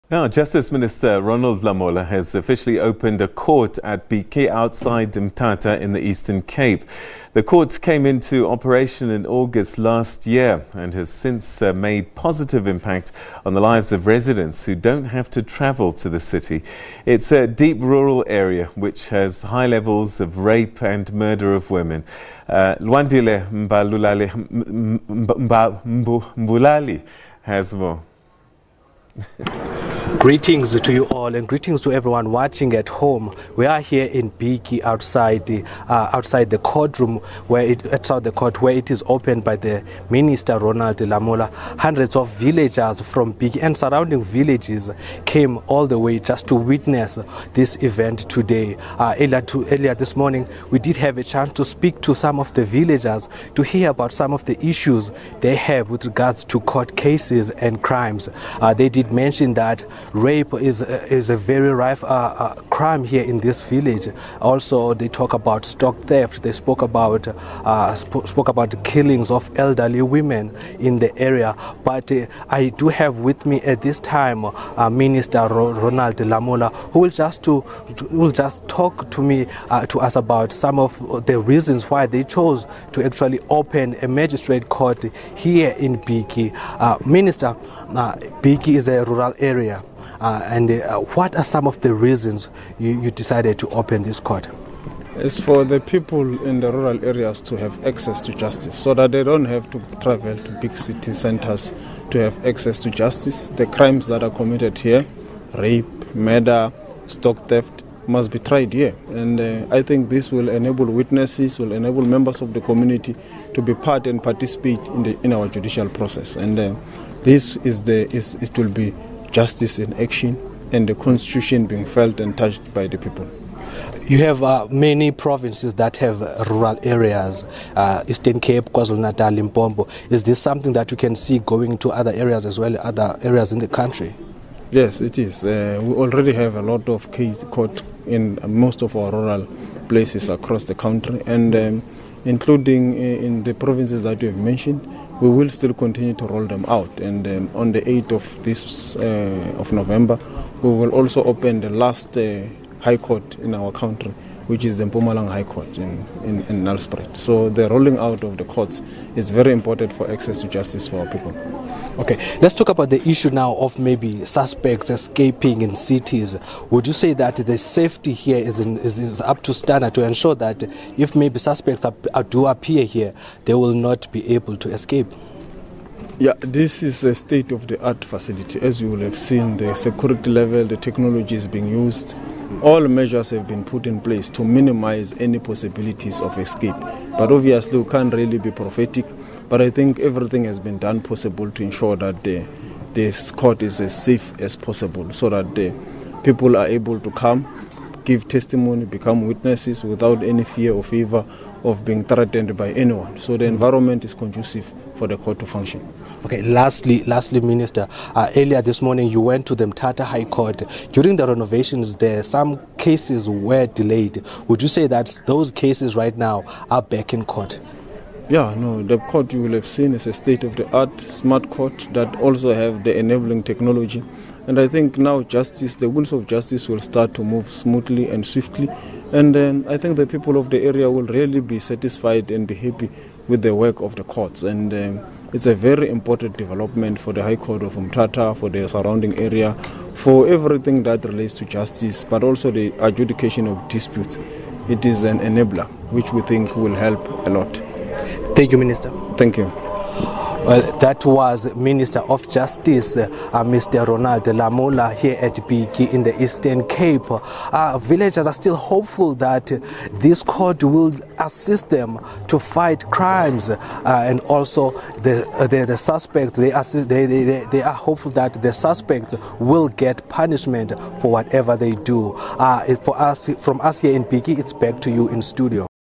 Bhityi court officially opened | Lwandile Mbulali reports
Justice Minister Ronald Lamola has officially opened a court at Bhityi outside Mthatha in the Eastern Cape. The court came into operation in August this year and has since made positive impact on the lives of residents who don't have to travel to the city. It's in a deep rural area which has high levels of rape and murder of women.
Lwandile Mbulali has more.